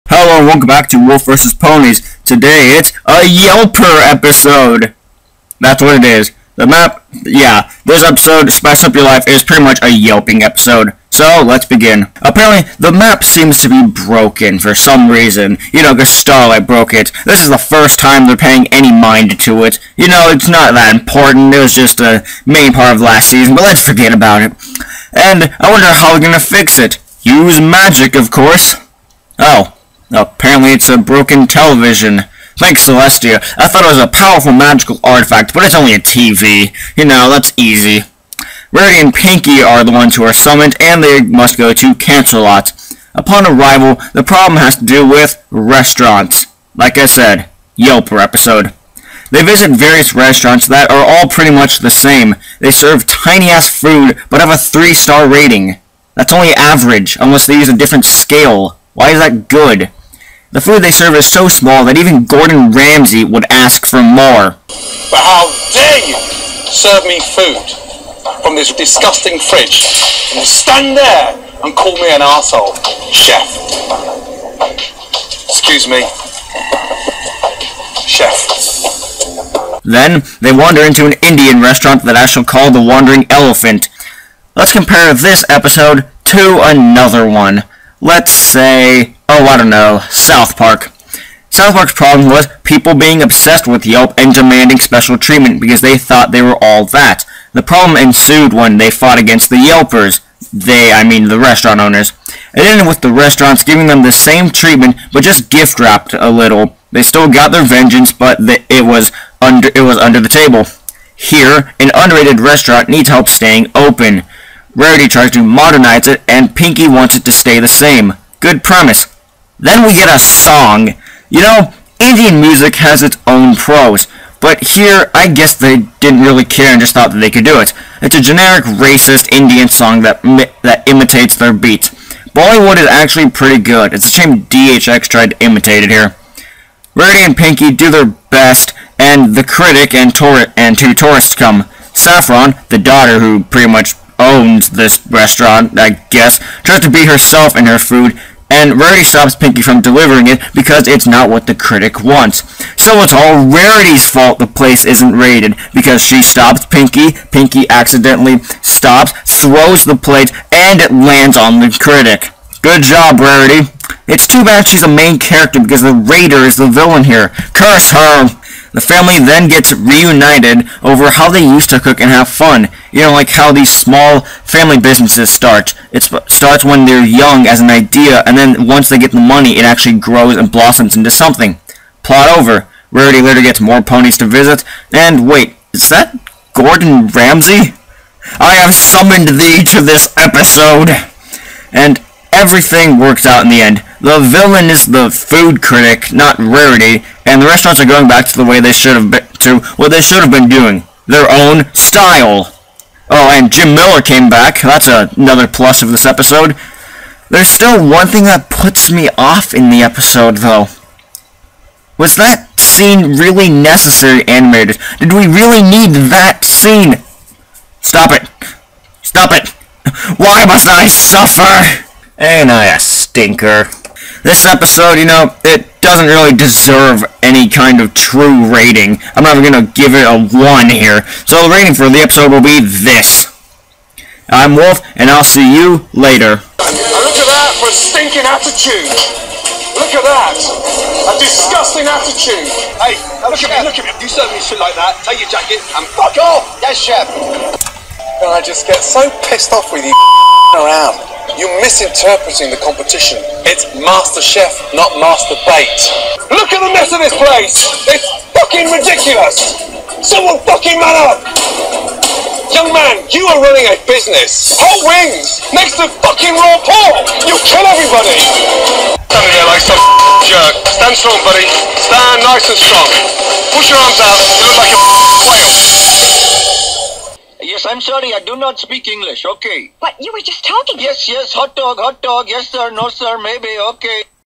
Spice Up Your Life Review
Wolf looks into the newest episode, about restaurants and appeasing food critics.